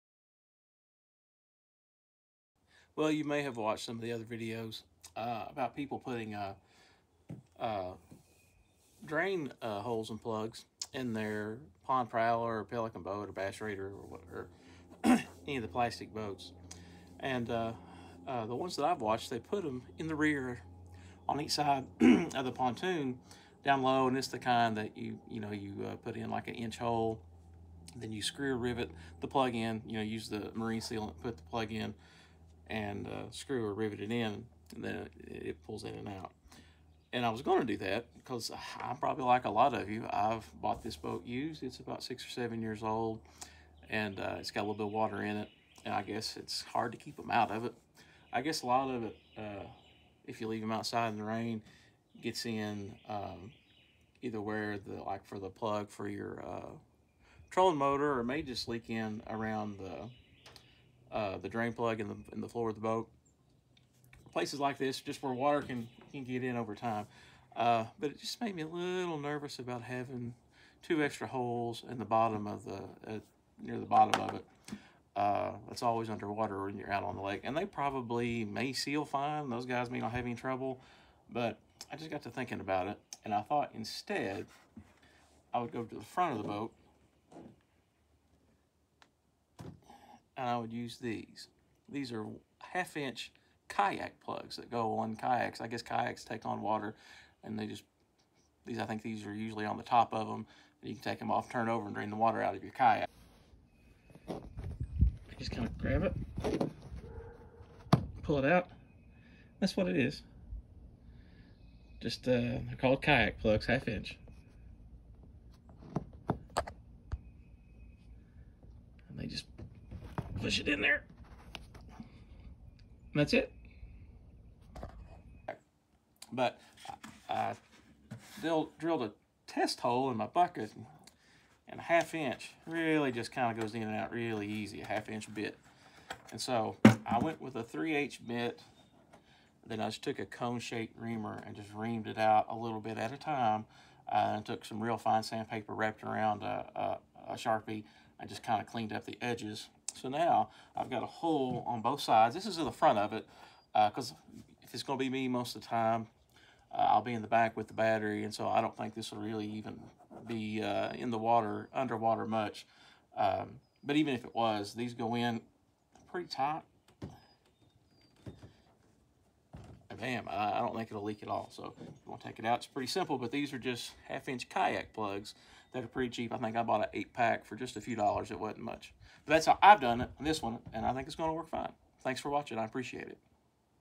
EASY Way to add extra drain holes to your Pond Prowler, Bass Raider, Pelican, or other plastic boat?
Here's my quick and fairly easy way to add drain holes and plugs to your plastic 2 man boat to drain the water out that works it's way inside the boat.  My method does require you to stand the boat up on end or at least close to it. But mine isn't on a trailer so it's not an issue for me.